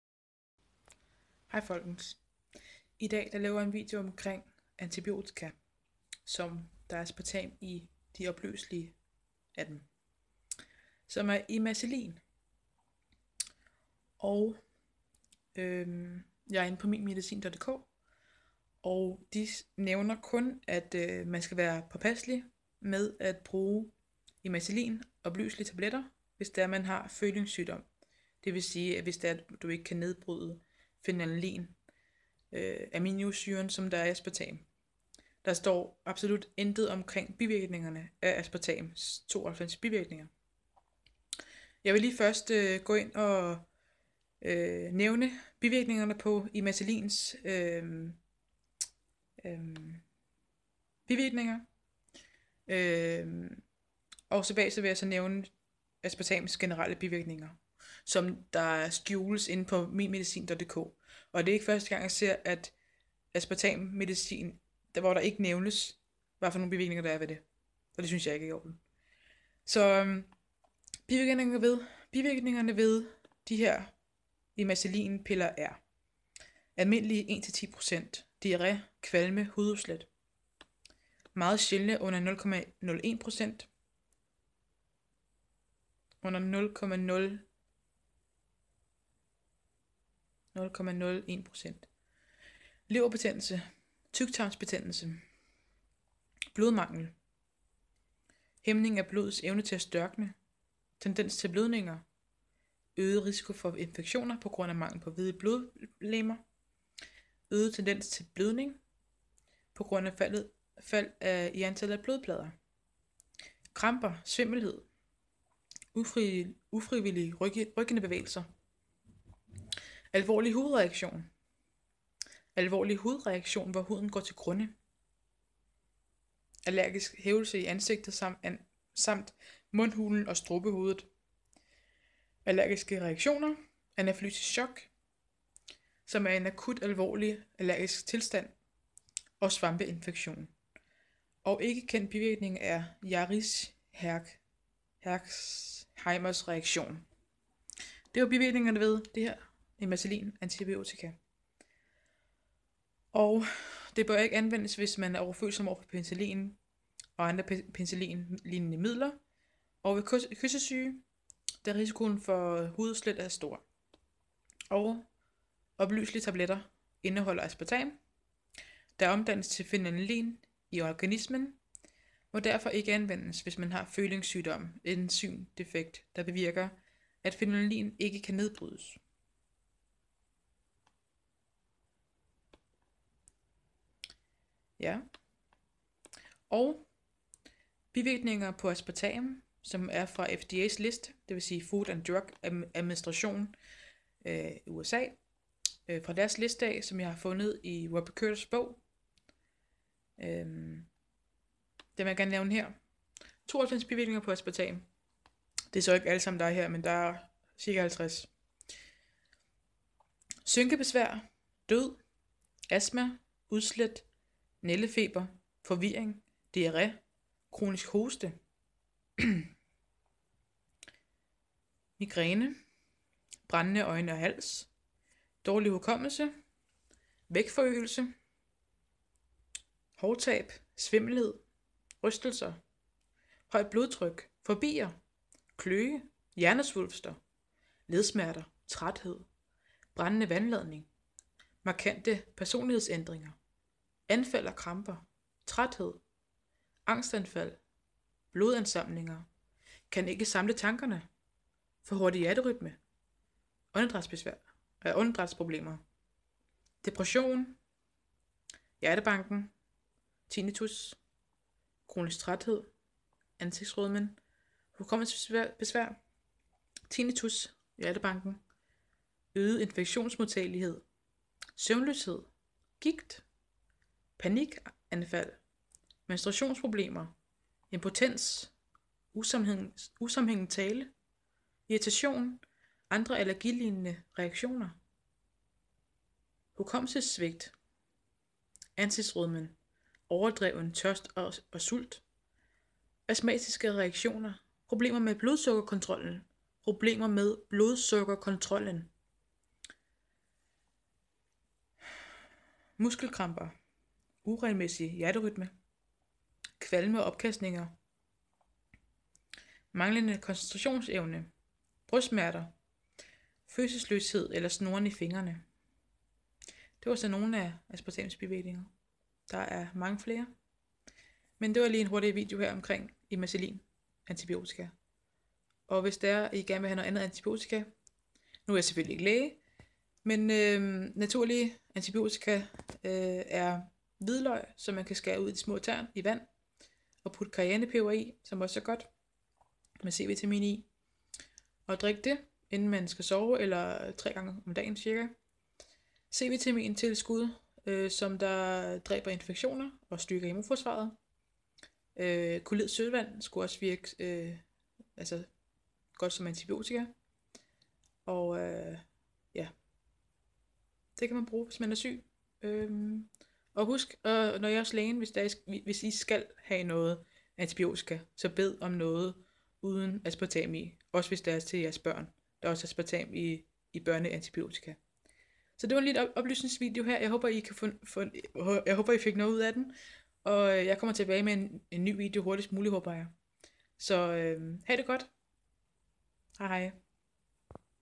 Antibiotika - IMACILLIN - opløselige tabletter aspartam
Aspartam medicin mangler ofte information om bivirkningerne ved aspartam, der står kun at man ikke bør anvende præparaterne hvis man har følings sygdom, en enzymdefekt hvor man ikke KAN nedbryde fenylalanin (aminosyre).